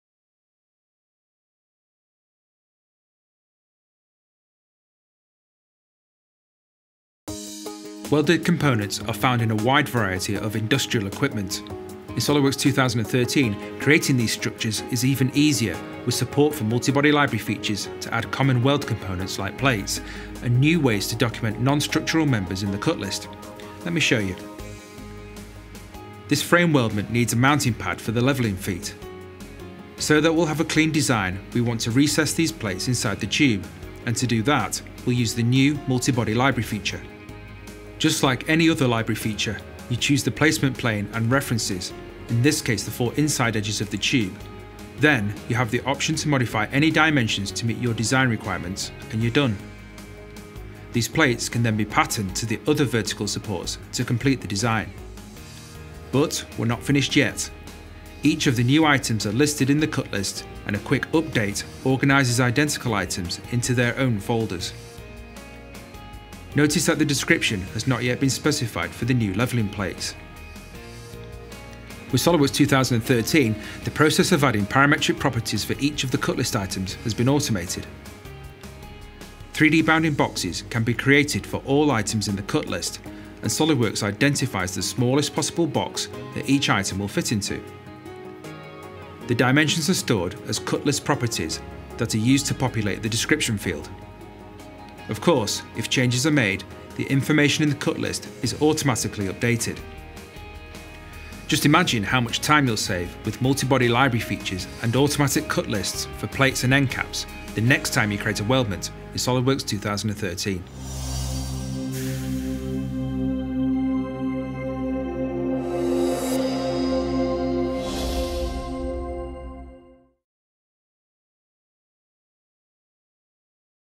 SolidWorks 2013: Weldments, Sheet Metal, and Multi-Body Library Features
Quickly design weldments, sheet Metal, and multi-body parts and increase standardization.

- Save time and automate outputs for manufacturing
- For Weldments—Access overall dimensions of the part bounding box for BOM, cut list, or annotations
- For Sheet Metal—Set and view insertion points for forming tools to determine the precise location of the forming tool relative to the part
- Multi-Body Parts—Use library features on multi-body parts

Get the full details about SolidWorks 2013 on our website--no registration required!